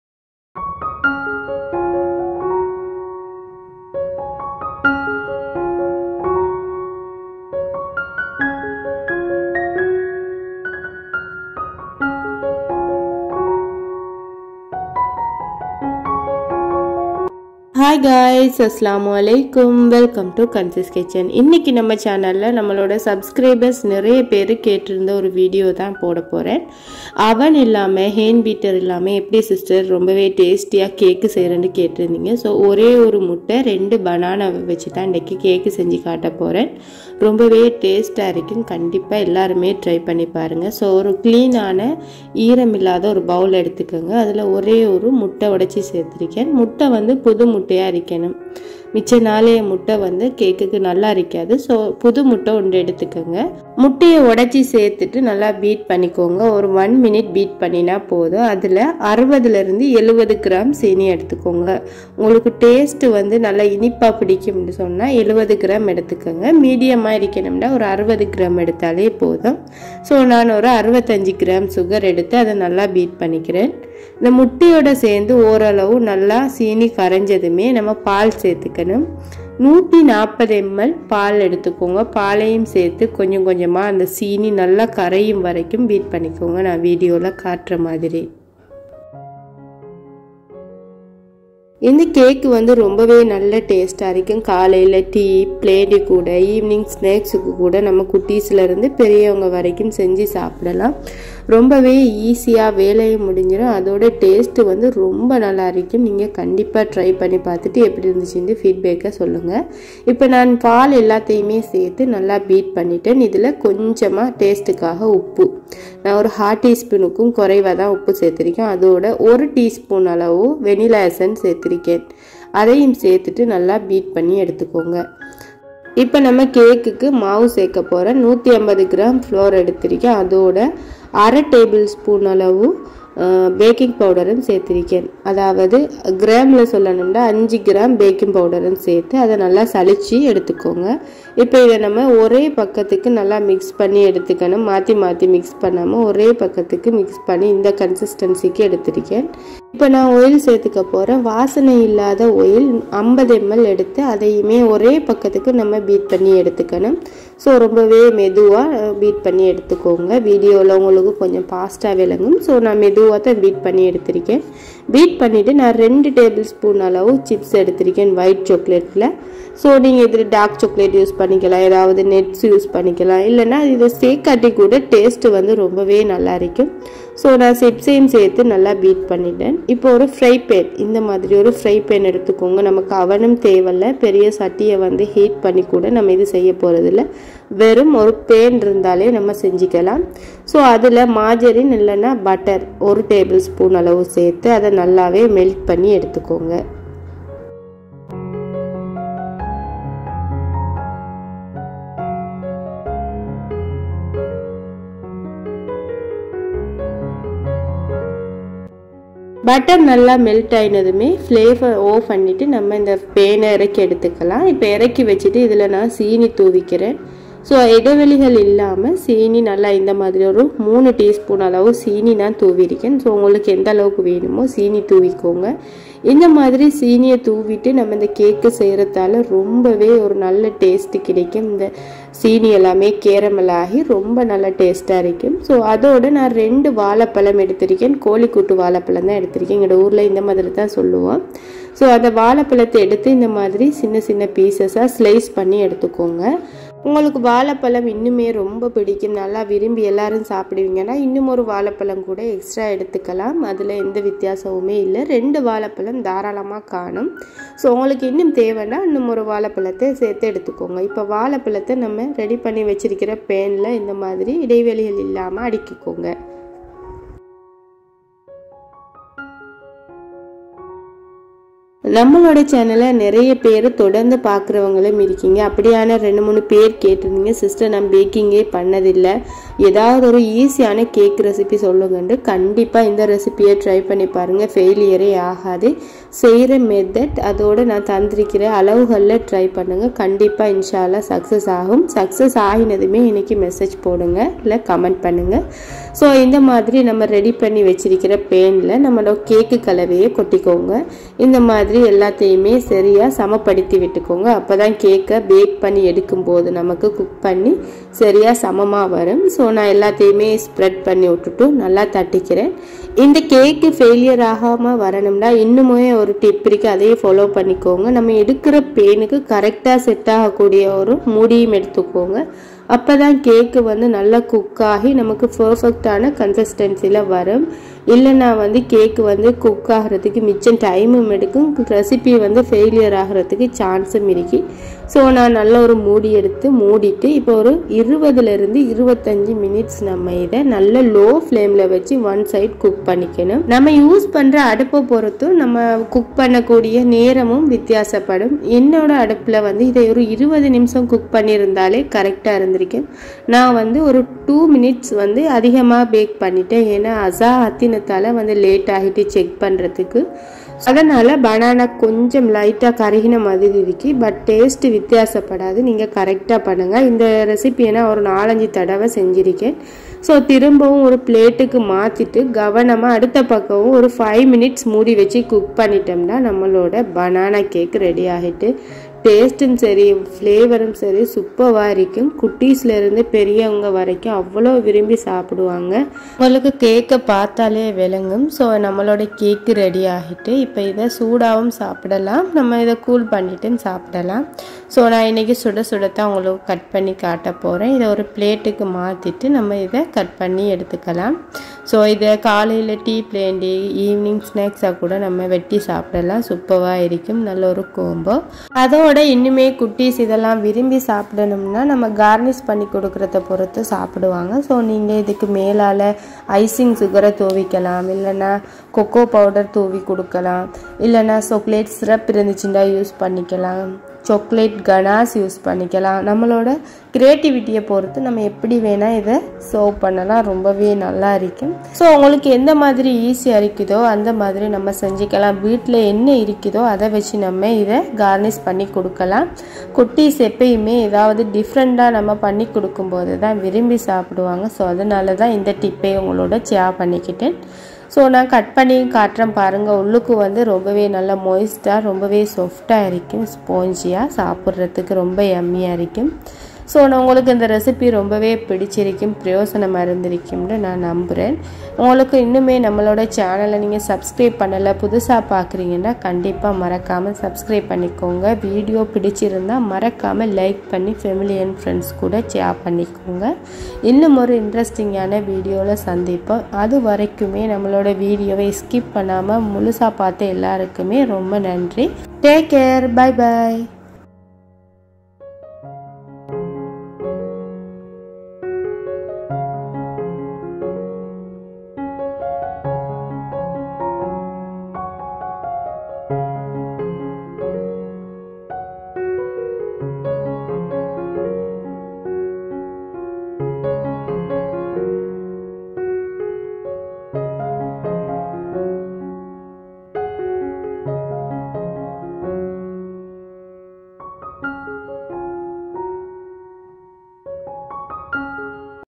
1 முட்டை 2 வாழைப்பழம் இருந்தா போதும்|BANANA CAKE|NO OVEN |NO HAND MIXER
egg 1
suger 60-70g
vanilla essence 1tspn
Salt
banana 2
flour 150g
milk 140ml
oil 50ml